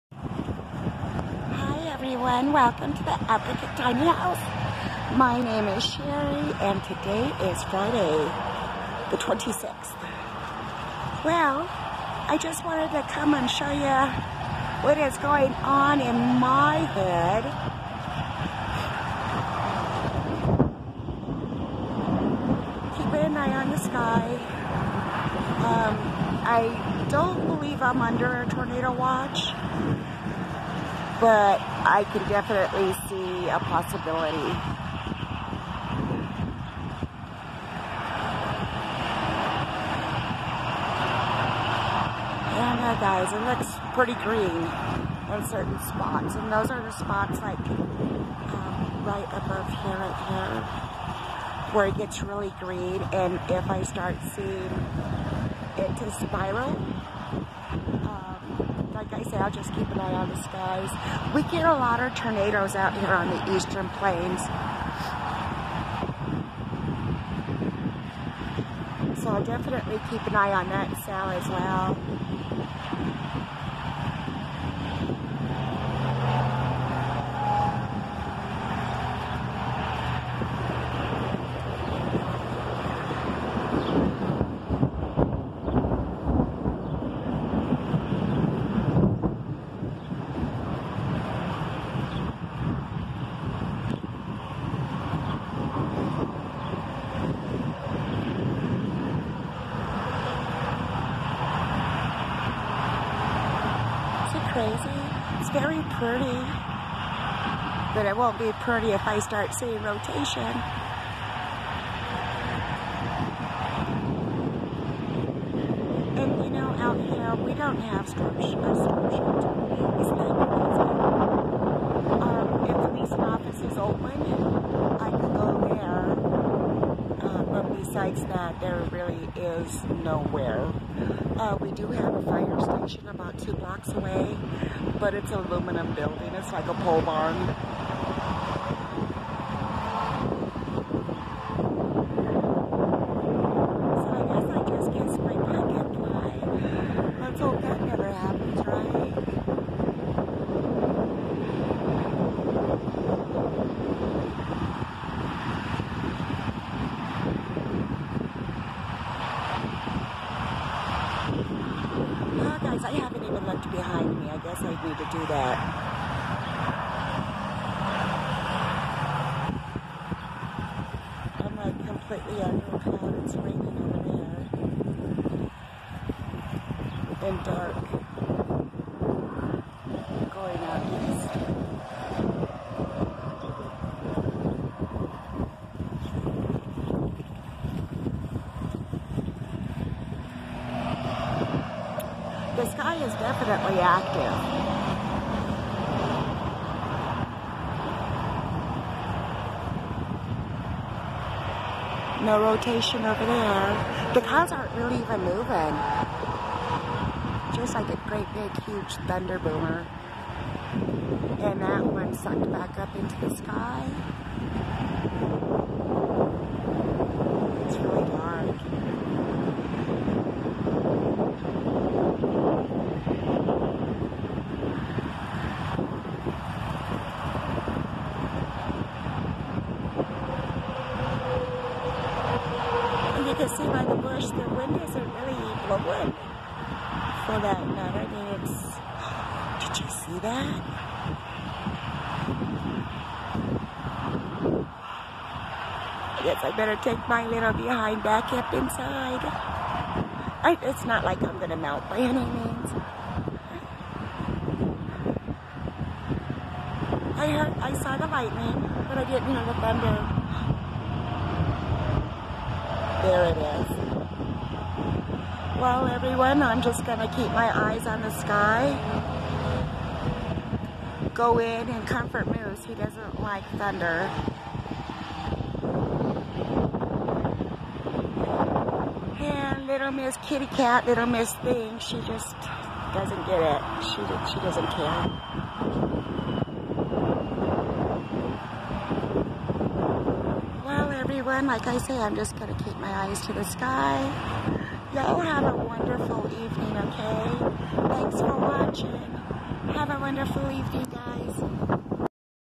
Twister Weather
Skies are looking pretty ominous . There is alot of green . The temperature today was 89 decrees with a dry heat .